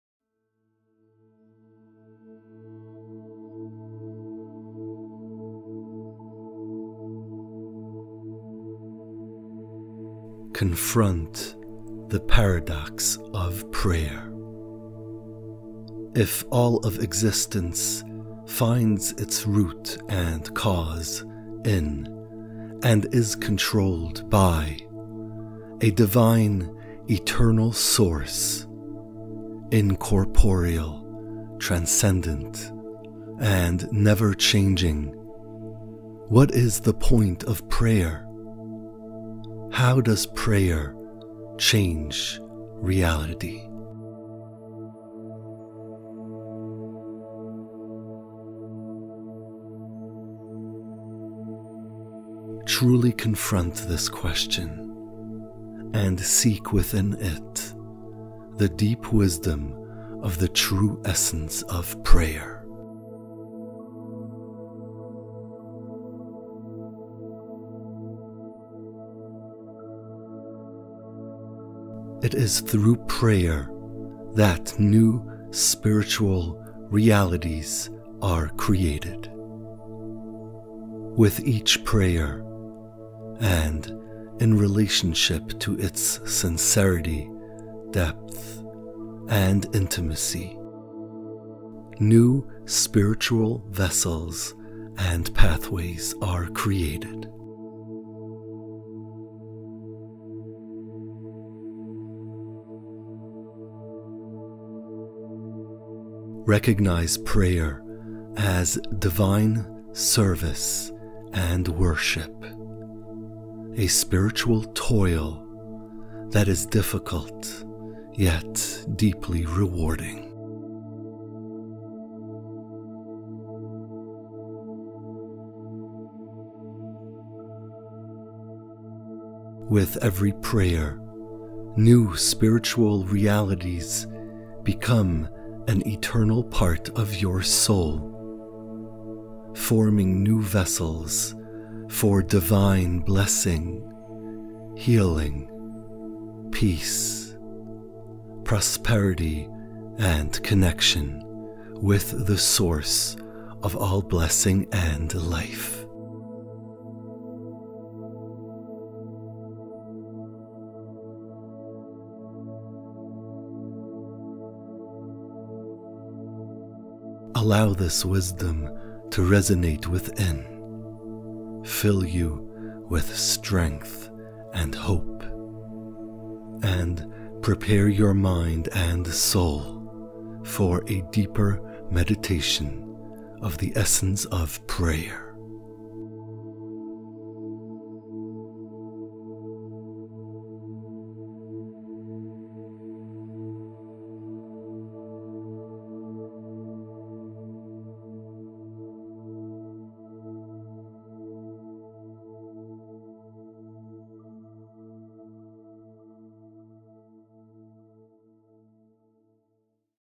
[85-4] Guided Jewish Meditations - Prayer Series: The Essence of Prayer
In the fourth of the series of short meditations on prayer, the paradox and essence of prayer is explored and meditated on. Through a deep understanding of the essence of prayer, the soul is ready for the last and final step.